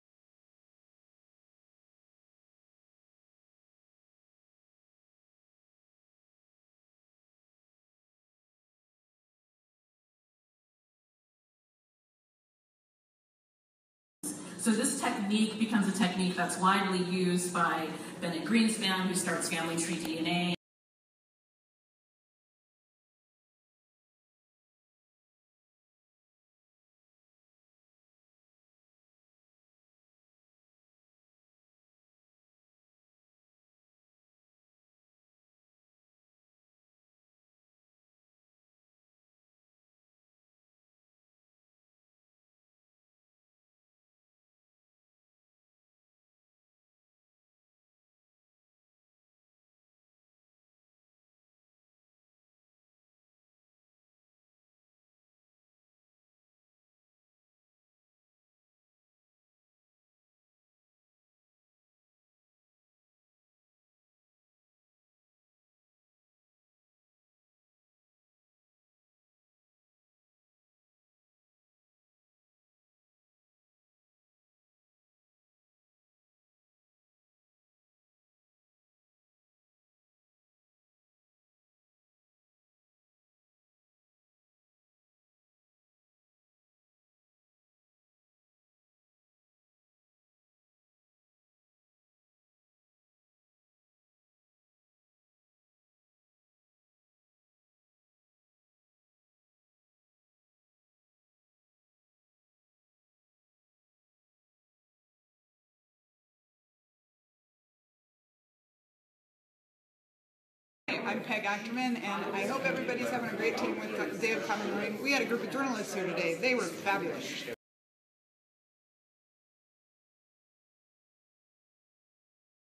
From Snapchat: Seattle Pacific University's 2016 Day of Common Learning
Dr. Alondra Nelson of Columbia University gave the keynote address at Seattle Pacific University's Day of Common Learning for 2017, "Tracing Our Roots, Telling Our Stories." After the afternoon sessions, theologian Miroslav Volf spoke about the role of faith in reconciliation.